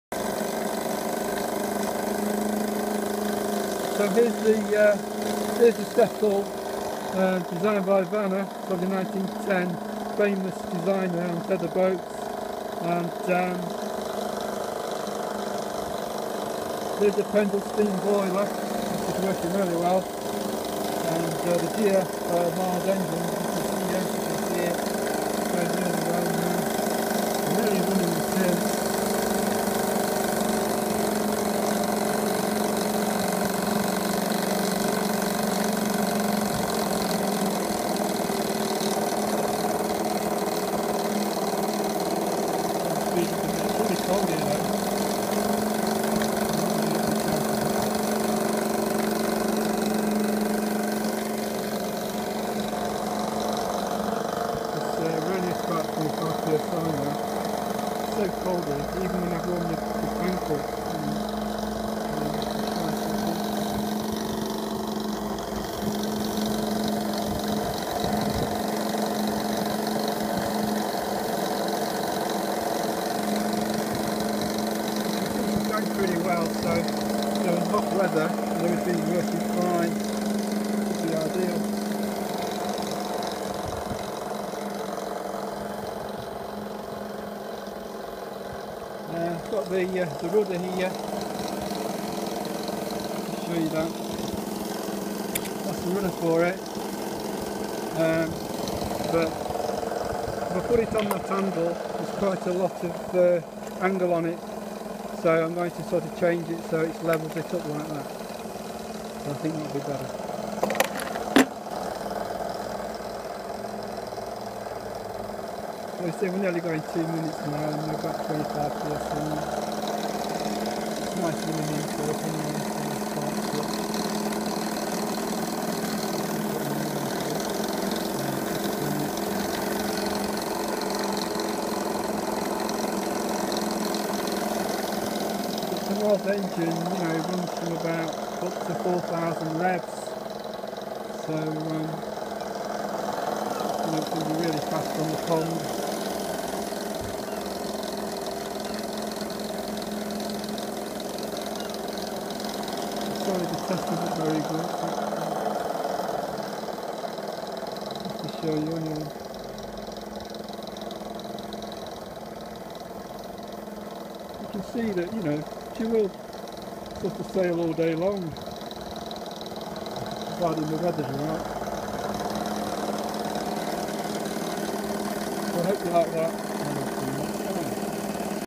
Vanna step tether hull G F Mars fast engine
the Tether Step hull dates to early 1900’s and attributed to Mr Vanna a famous engine and model boat designer  .
 The GF Mars engine is high speed engine  ratted at 4000 rpm
 This bench test is still in working progress ,. Still jobs to do her but
 Getting there , hope to sail this summer .